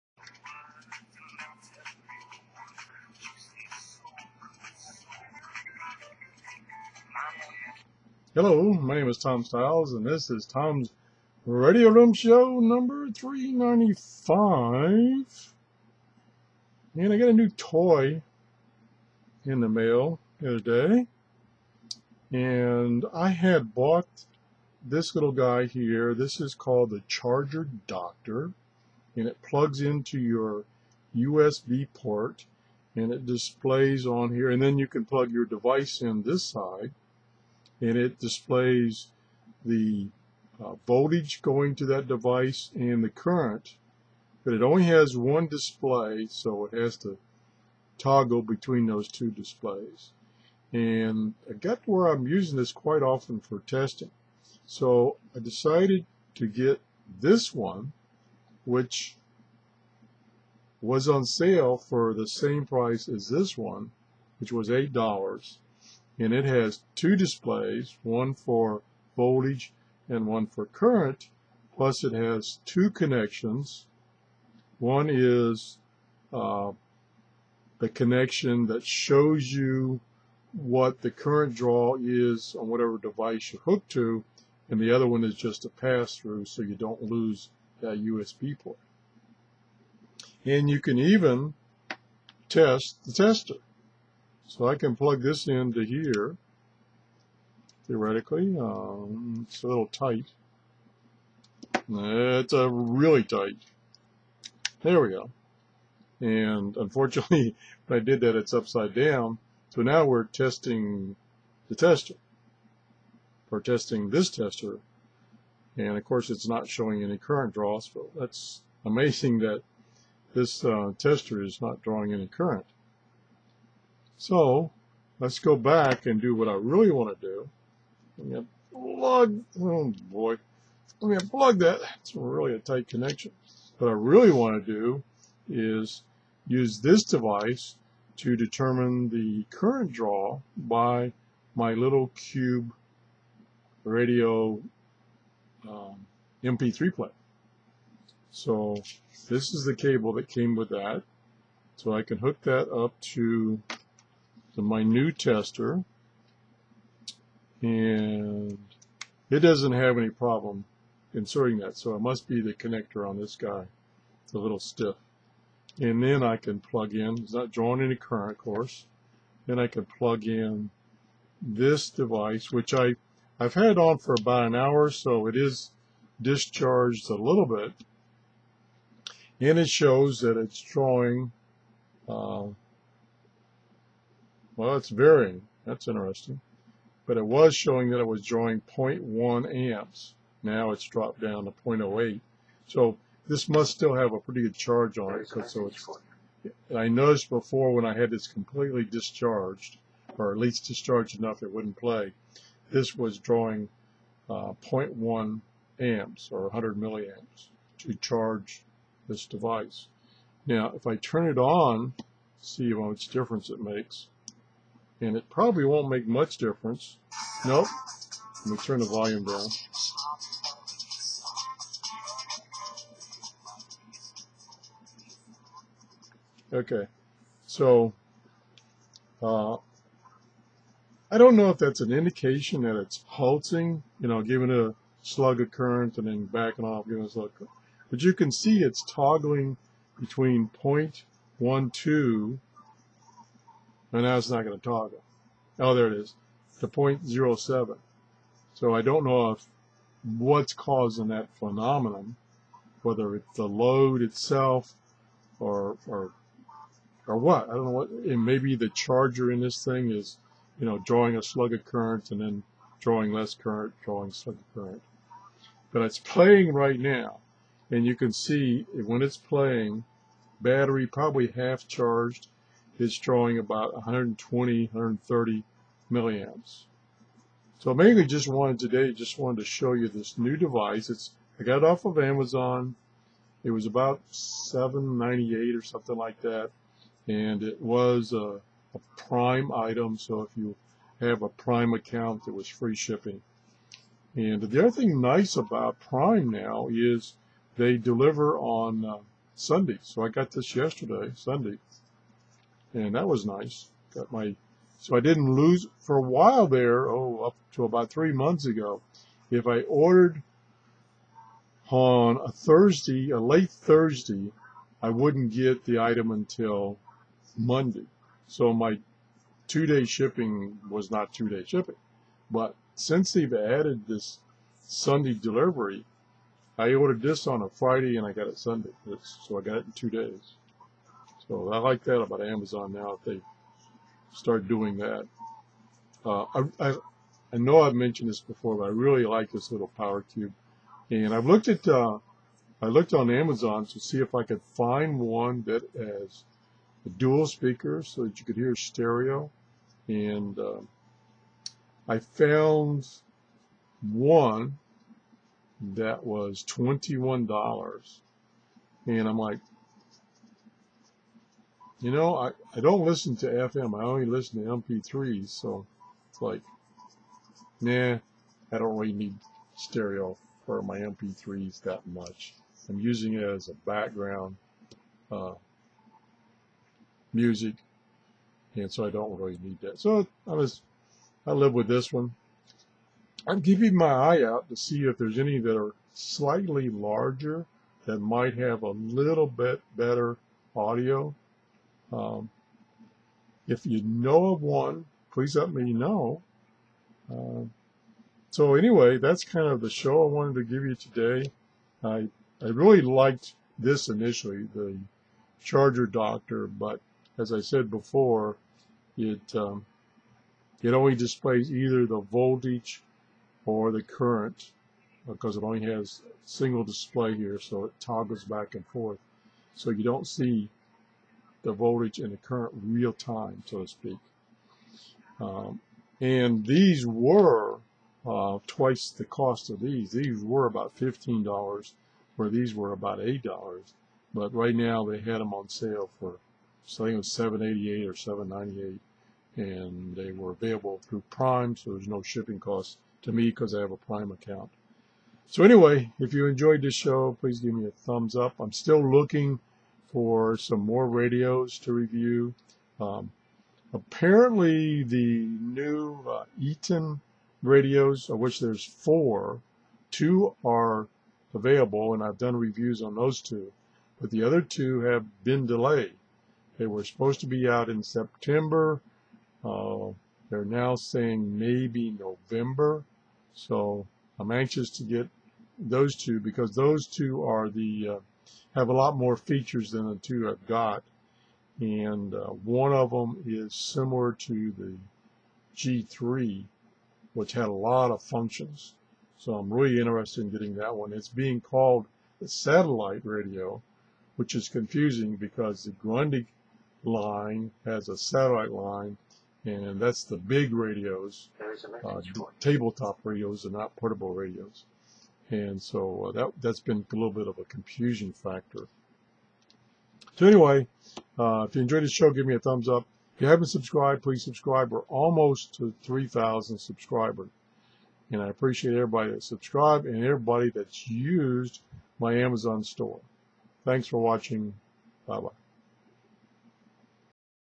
TRRS #0395 USB Tester with Dual Display
USB Tester with Dual Display testing my Mp3 player cube. Note some of these come with a Red/Red display.  This has a Red/Blue display.